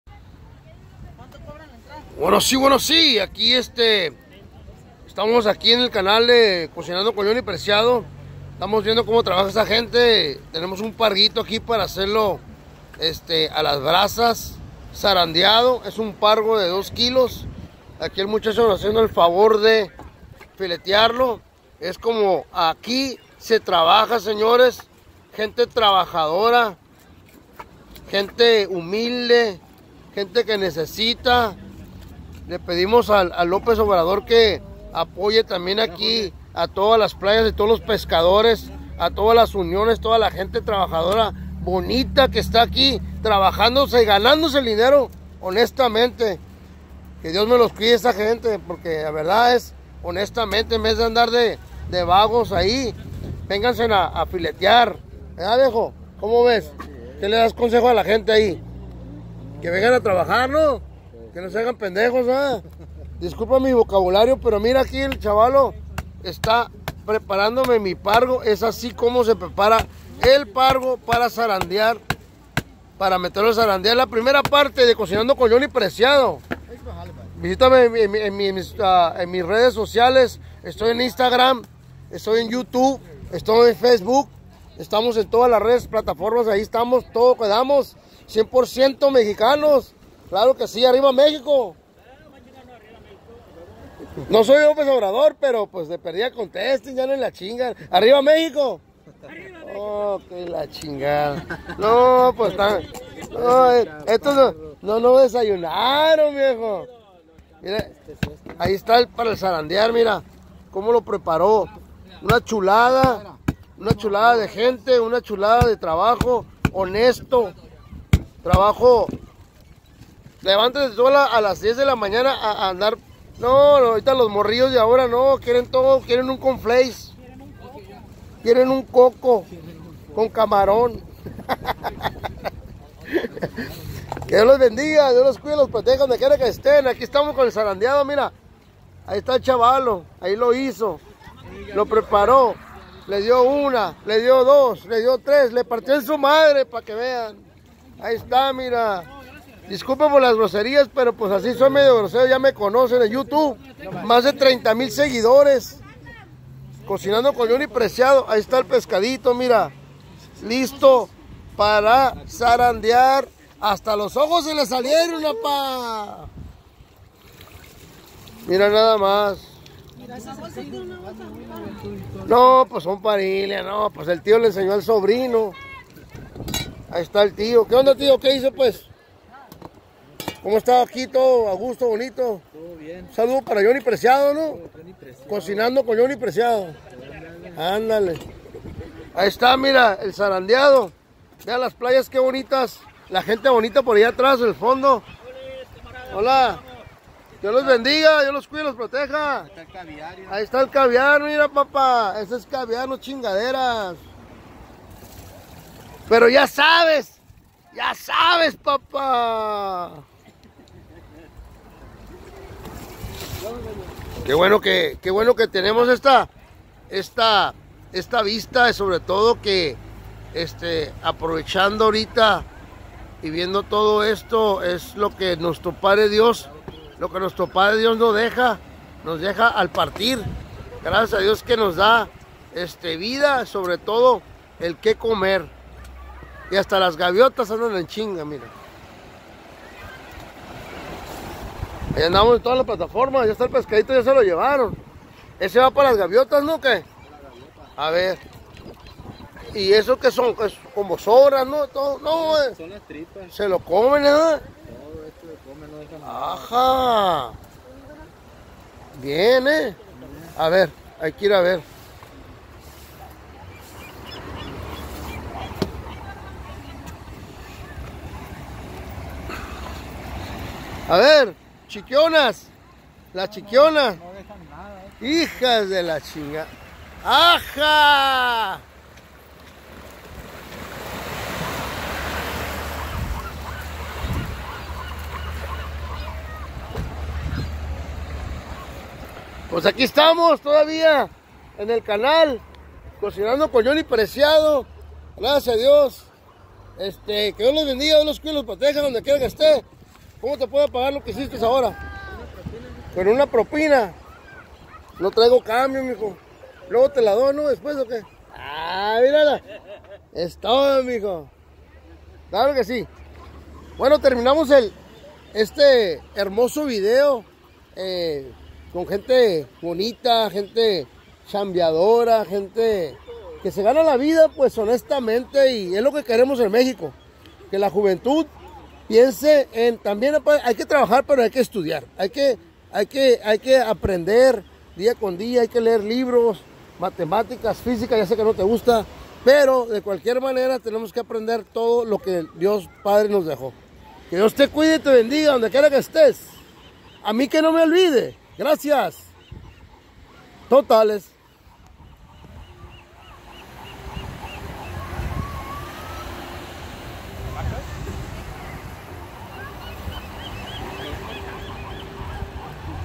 SARANDEADO DE PARGO 1RA.PARTE,COCINANDO CON JOHNNY PRECIADO
SARANDEADO DE PARGO 1RA.PARTE,PREPARADO DE PARGO,COMO ABRIR TU PESCADO PARA SARANDEAR,AJO,MOSTAZA,CEBOLLITAS ,PIMIENTA,AL CARBON DE MANZANITA.